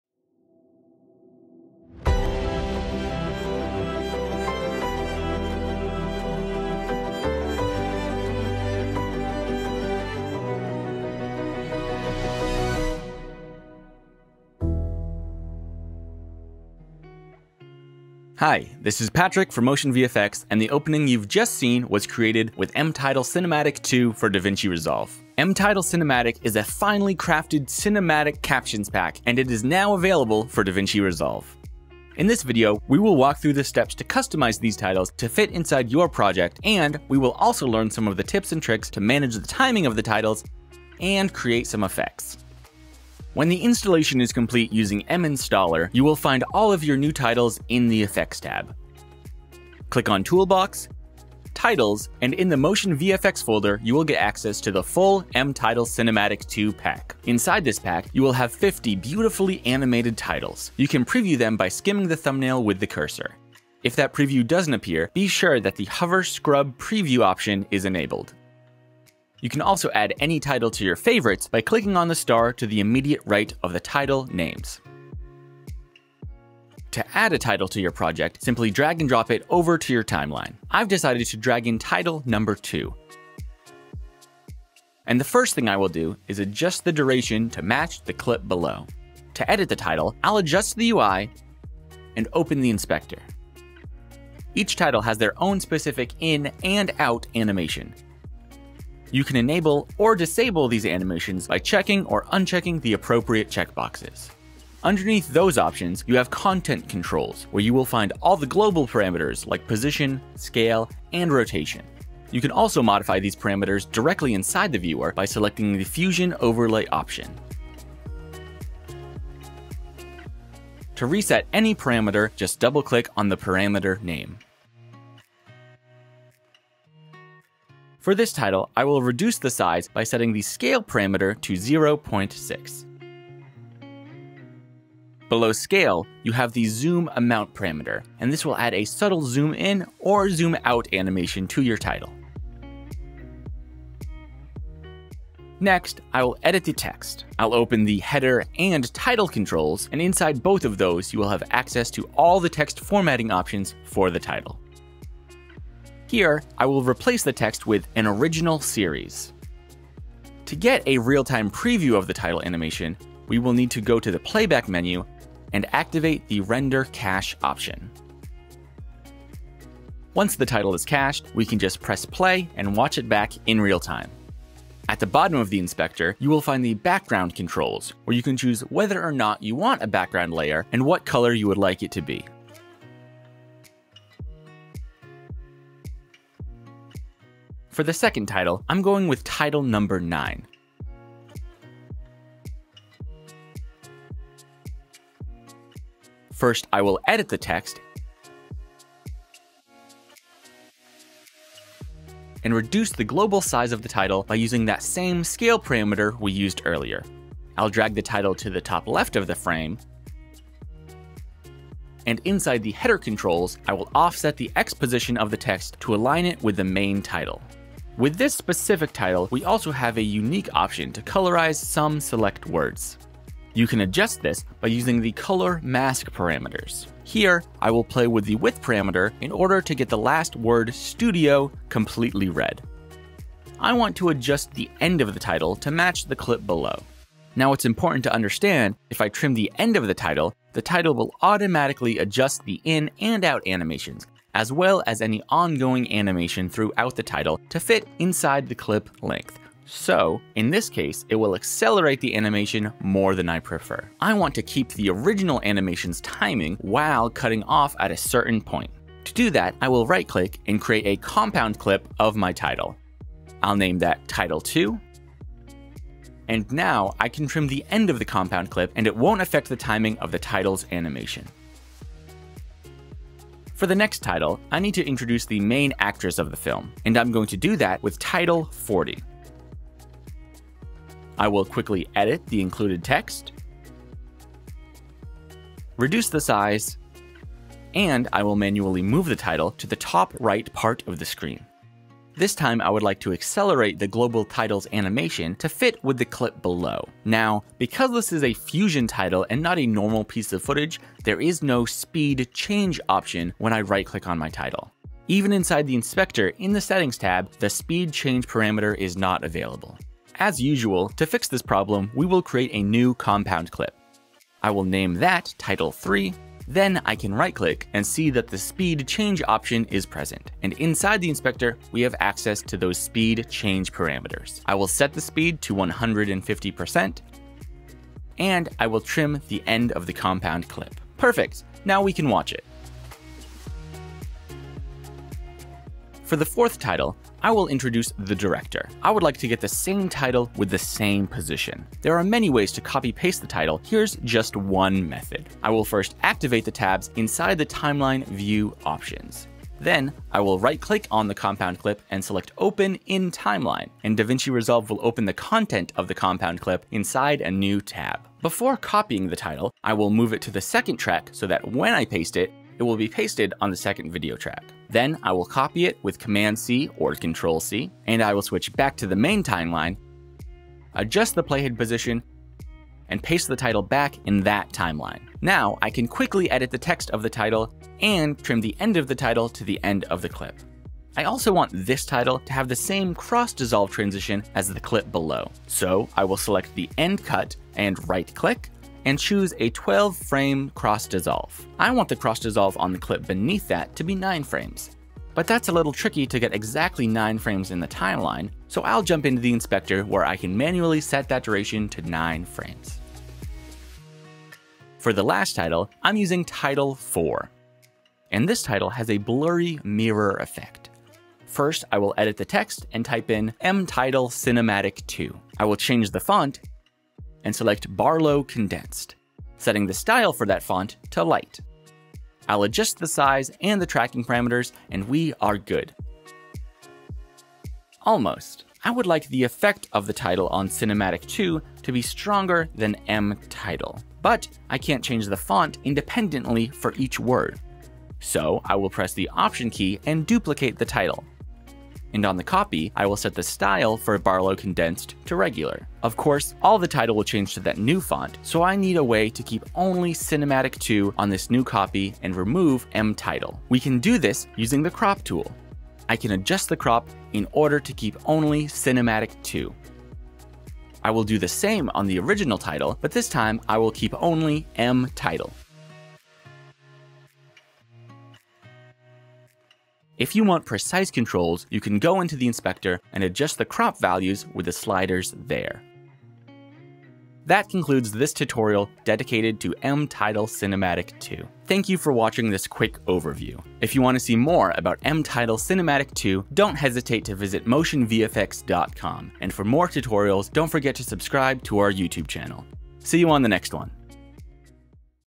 mTitle Cinematic 2 DVR Tutorial — Using, customizing cinematic titles in DaVinci Resolve — MotionVFX
In this tutorial, Patrick Stirling will teach us how to customize the titles from the new pack mTitle Cinematic 2 for DaVinci Resolve. We will get a complete overview of the parameters, tips and tricks concerning the workflow, and ways we can manage the titles' timing inside DaVinci Resolve.

00:00 The opening title project
00:18 Introduction
00:43 Locating the content in the effects tab
01:16 Adding and customizing Title 02
03:07 How to cut a title without modifying the timing
04:33 How to accelerate a title
05:35 Copy / Paste titles inside compound clips
06:44 How to apply 2 different fonts to 1 title
07:57 Conclusion